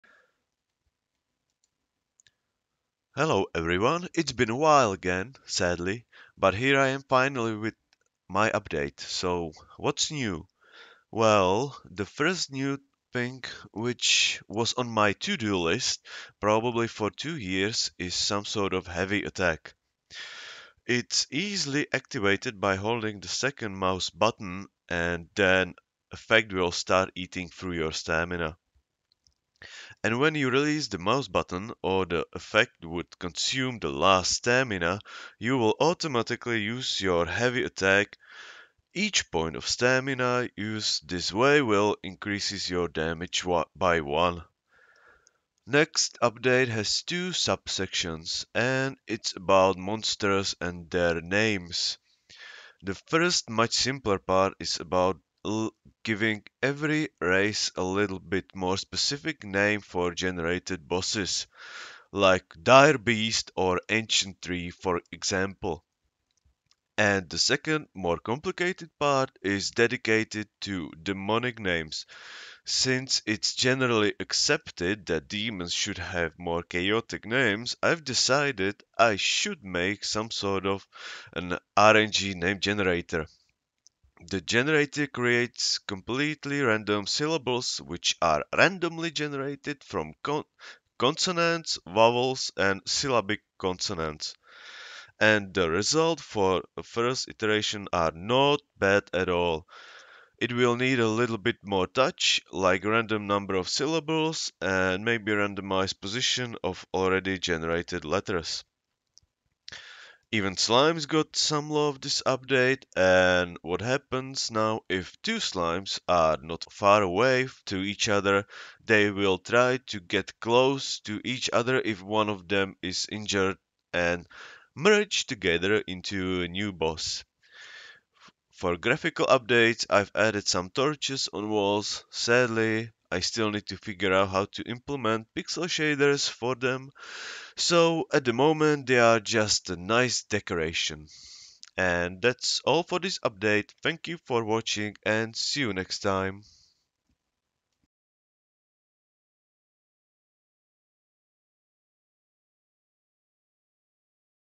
Game Update 0407: Smash, Torch and Random Names
Used software: OS - Manjaro 64
Text Editor - Atom
Game Engine - Love2D
Video Capturing - SimpleScreenRecorder
Video Editing - Kdenlive
Audio Recording - Ardour
Raster editor: Gimp

News:
- Heavy attack: Holding the right mouse button means that you will start loosing stamina. Each point of stamina used this way will increase your damage by one.
- Monster name generator: Each race has a different prescripted name to increase variation. Demons have randomly generated names.
- Slimes AI: If 2 slimes are close ot each other, they will try to merge if one of them is hurt
- Torches as graphical effect